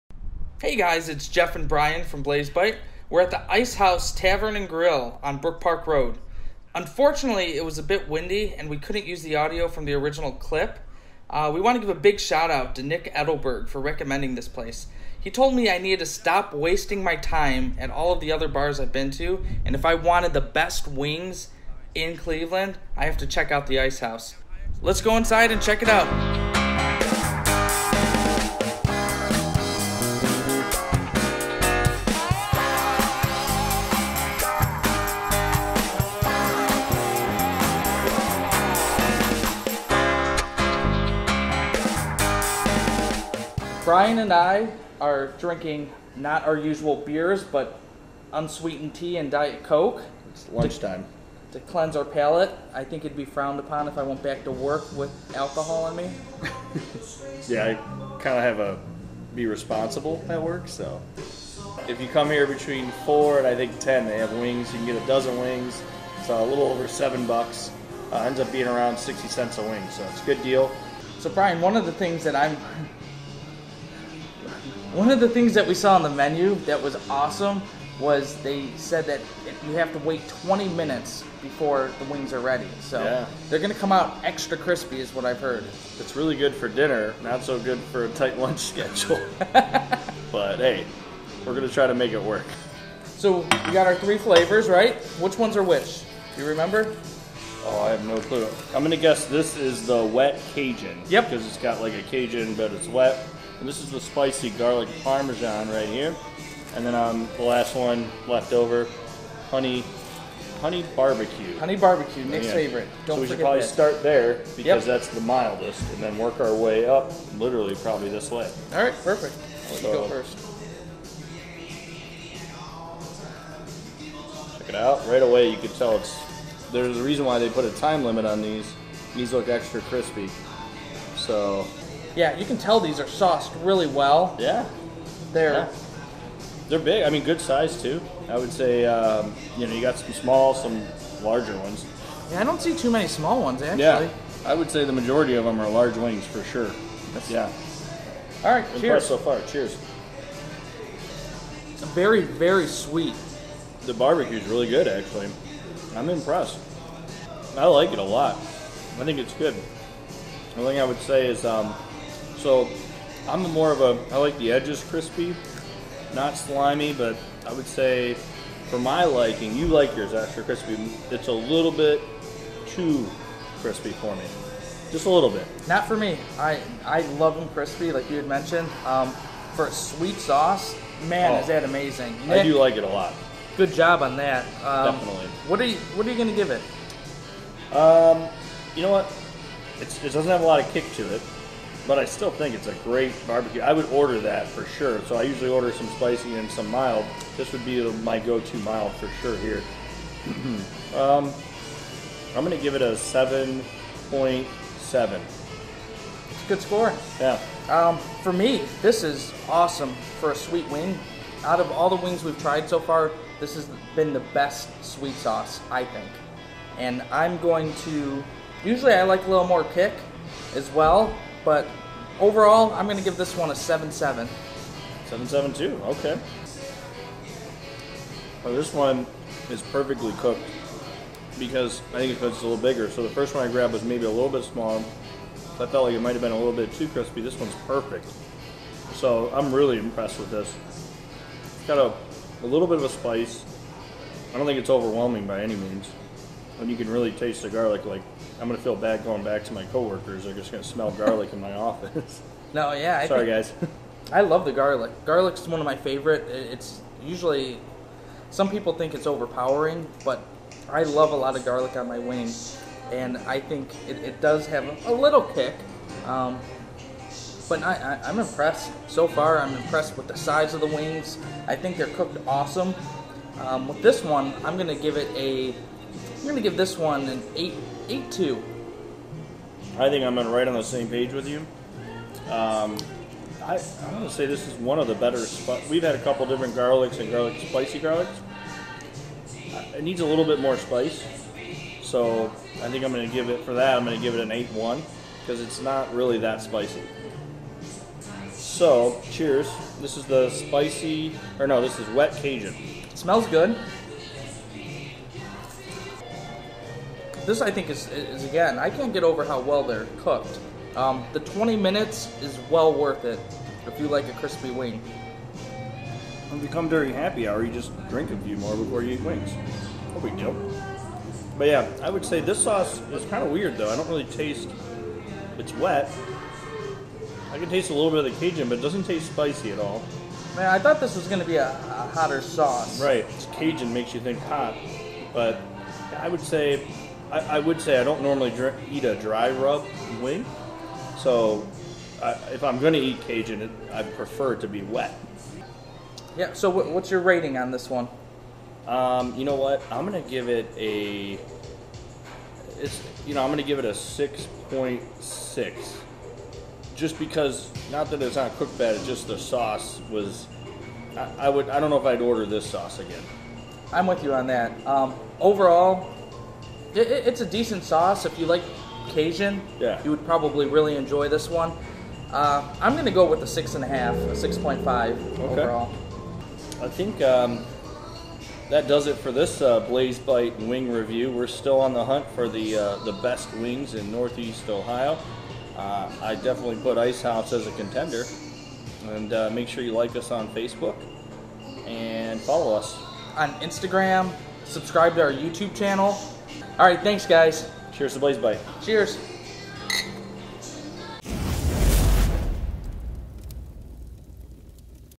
BlazeBite Wing Review - Ice House Tavern (edited)
Jeff and Bryan are on a mission to find the best wings in Northeast Ohio! This week they visit the Ice House Tavern & Grill in Brooklyn.